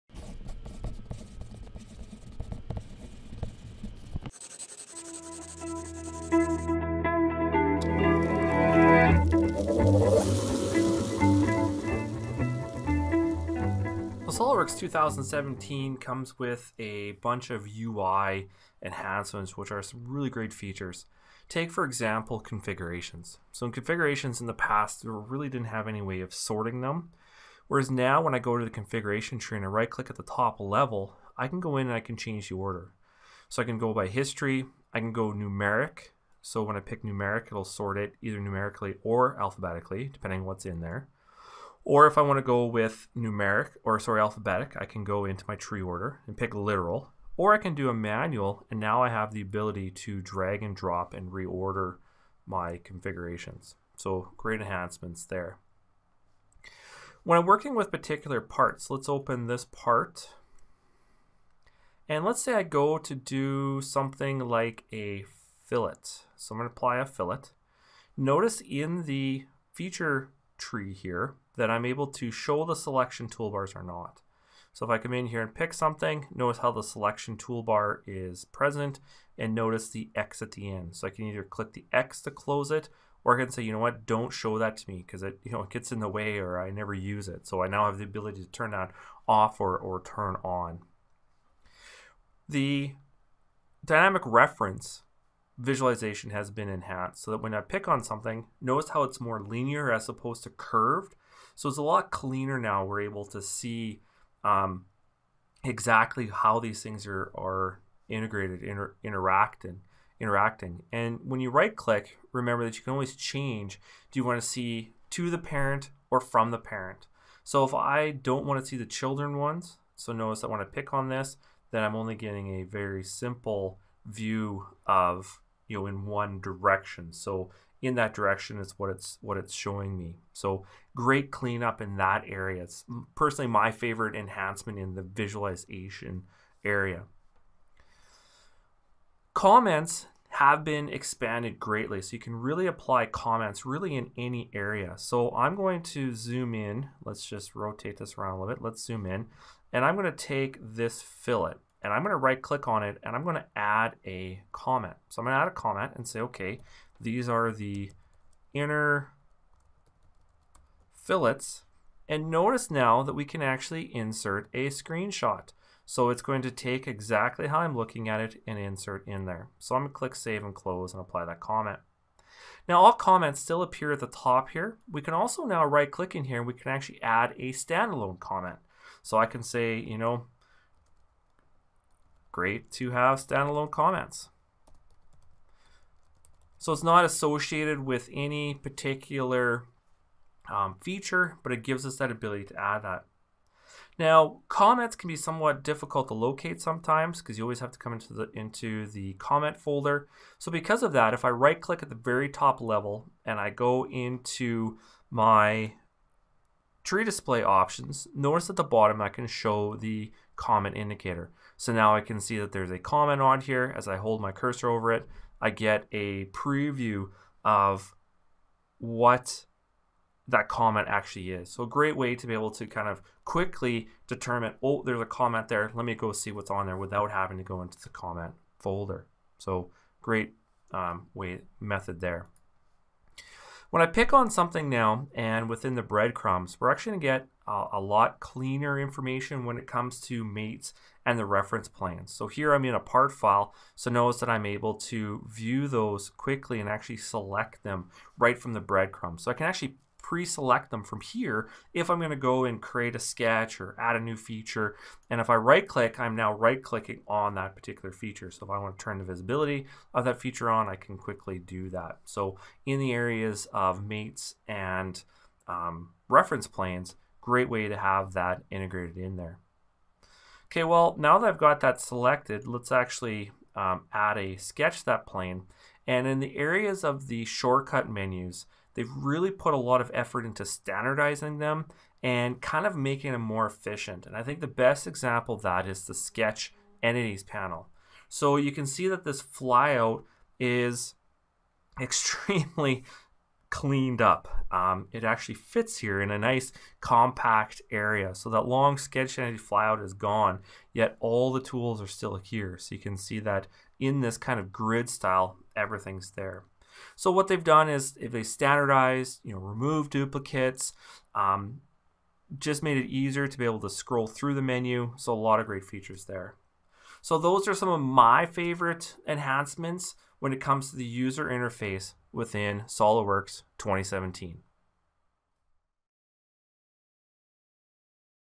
SOLIDWORKS 2017 User Interface Enhancements
A look at the user interface (UI) enhancements in SOLIDWORKS 2017